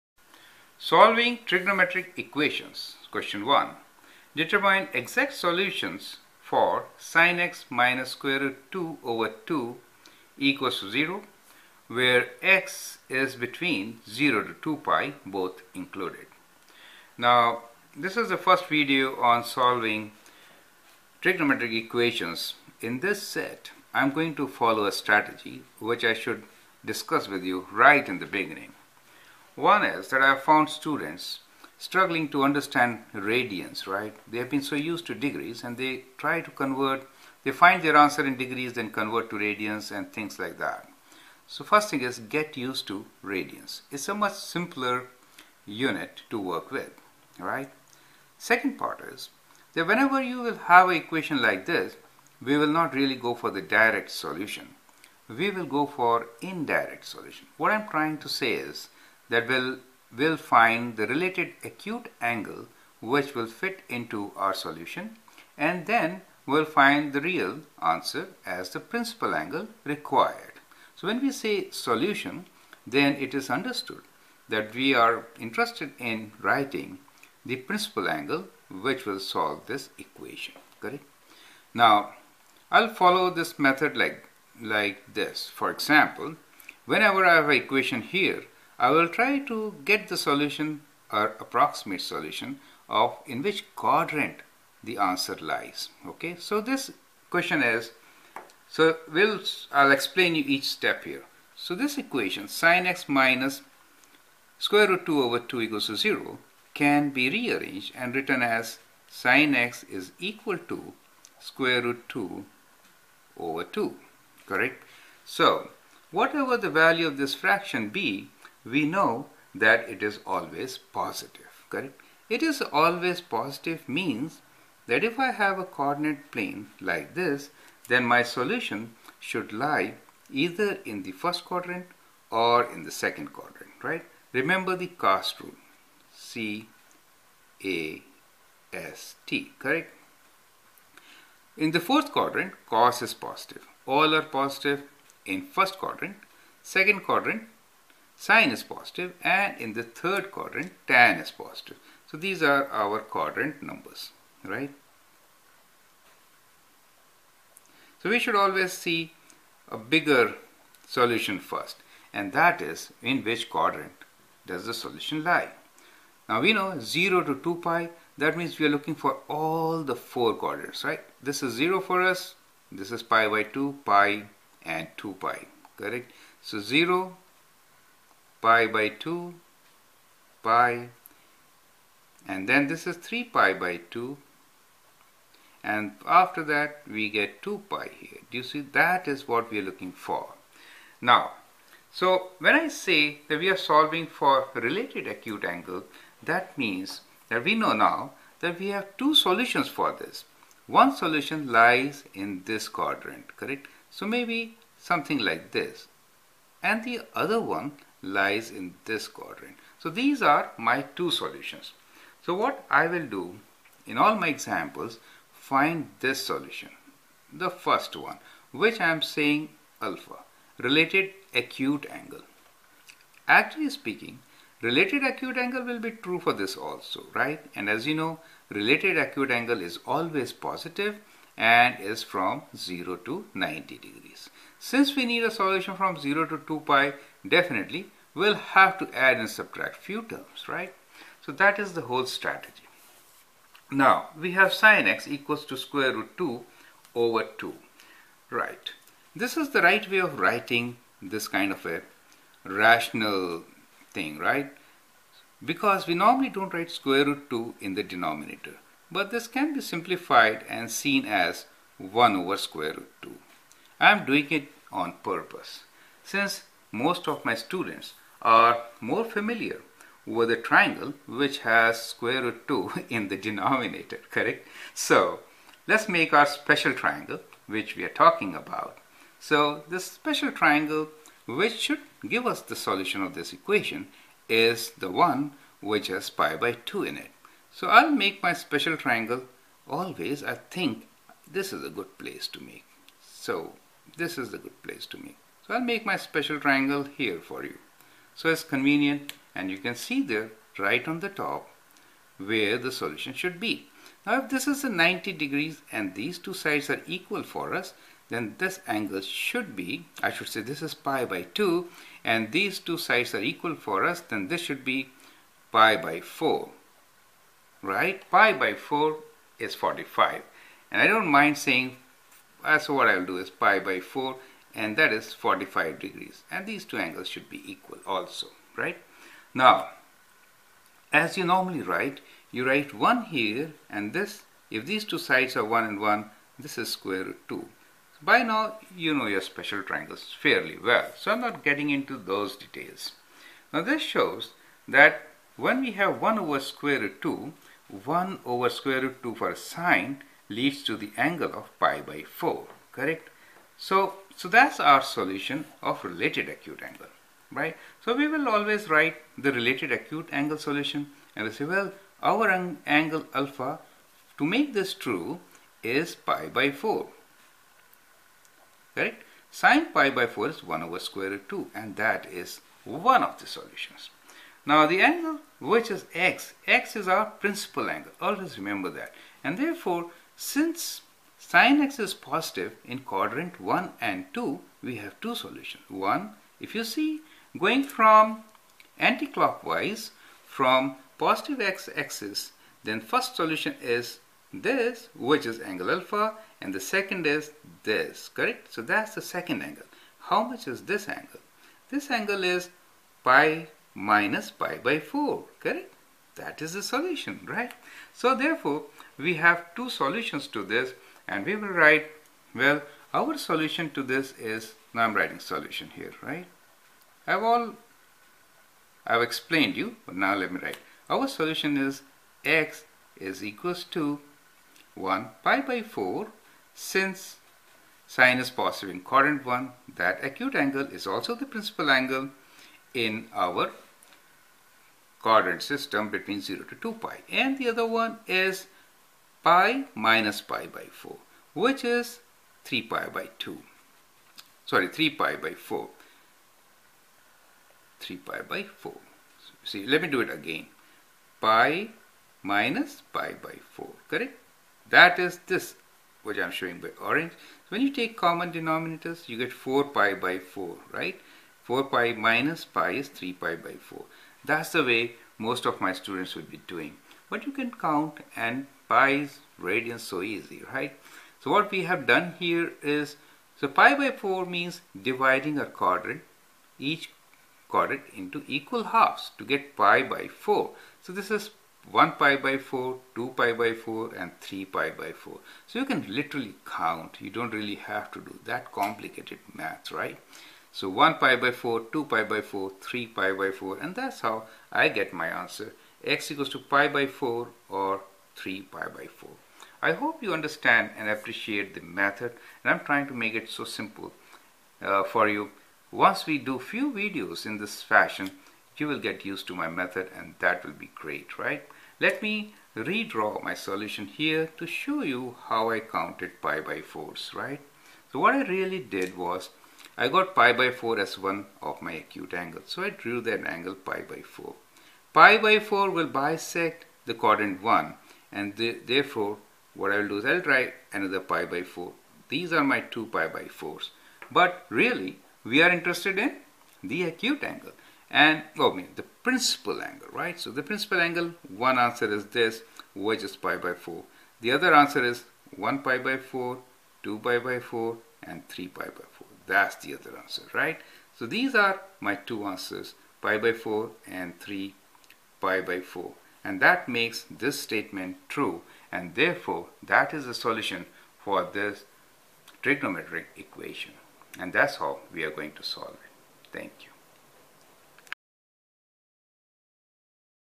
Trigonometric Equation Exact Solution Sine Q1
Find related acute angle and the quadrant to get all the possible solutions to the trigonometric equation.